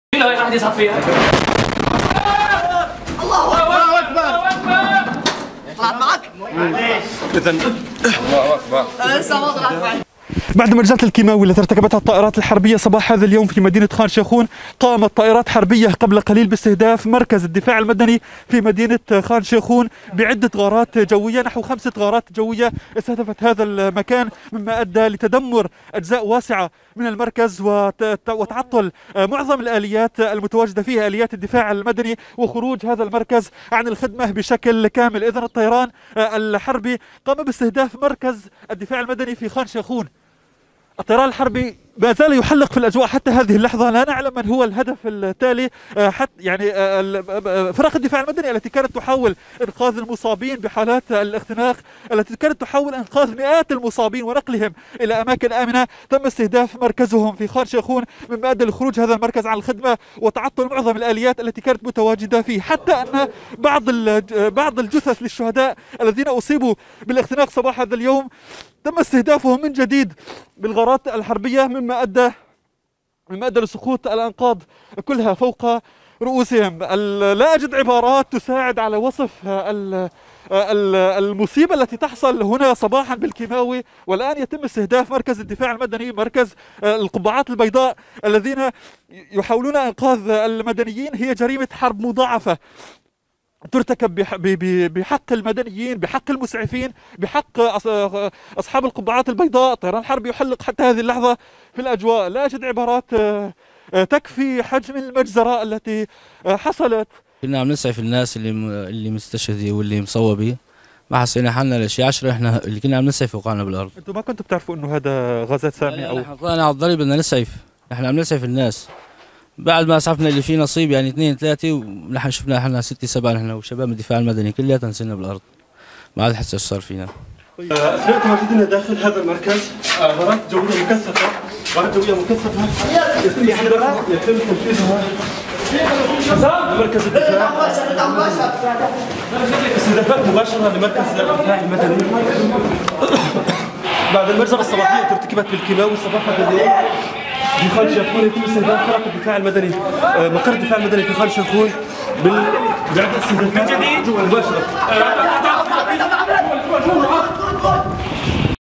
Targeting the Civil Defense Center during the rescue of the injured in chemical weapons attack
The Civil Defense Center was targeted during the rescue of the injured people suffering from suffocation after targeting Khan Shikhon city with poisonous gases, killing more than 70 people including children, and injuring around 250 others.
Khan Shikhon center is out of service, and the injured were transferred to other nearby points.

Report by: Hadi Abdullah | Khan Shikhon city, Idlib countryside | 4 Apr 2017